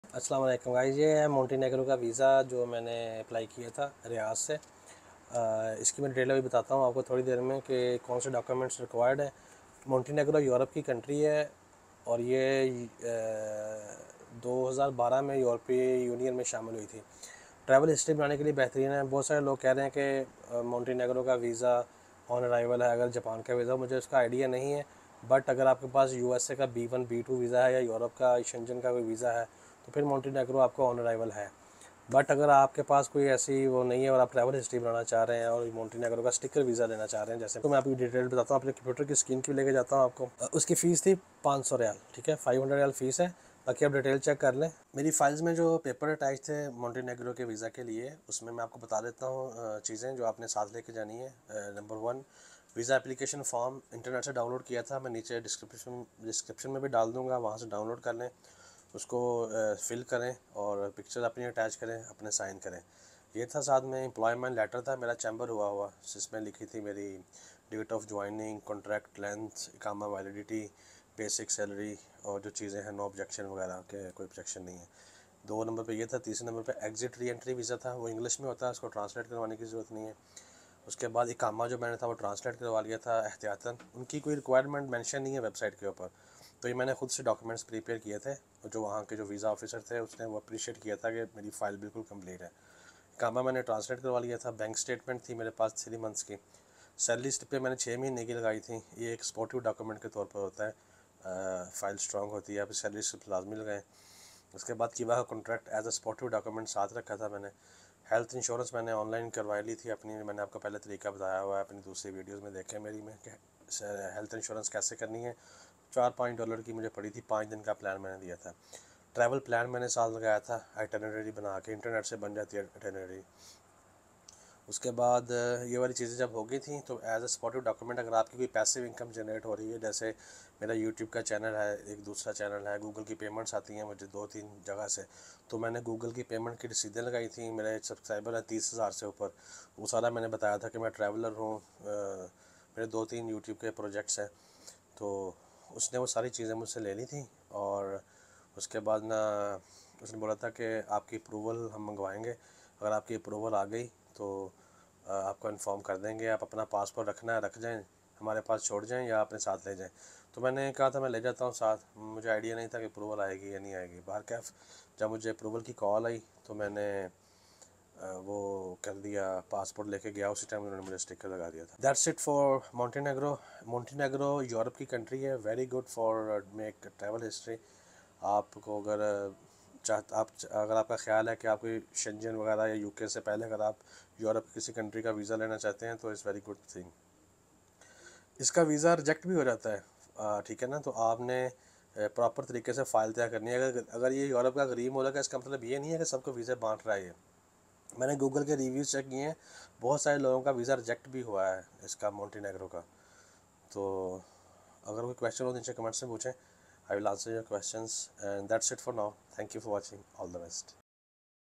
How to Get Montenegro Visit Visa from Saudi Arabia?
In this video we will discuss about Montenegro Tourist visa! I got my Montenegro visa from Saudi Arabia in 25 days. Watch the complete video and follow the checklist if your next destination is Montenegro.
Montenegro is an European country and its a parted from Serbia and got its European membership in 2012.

Hope this video will help you to get an European countries visa.

Documents Checklist:
1. Visa form duly signed
2. Passport size photo
3. NOC from employer chambered
4. Bank statement
5. Hotel booking
6. Return ticket
7. Travel plan
8. Iqama translation
9. Health insurance
10. Passport Photocopy and Original

How to Get Montenegro Visit Visa from Saudi Arabia? The Bilal's Show!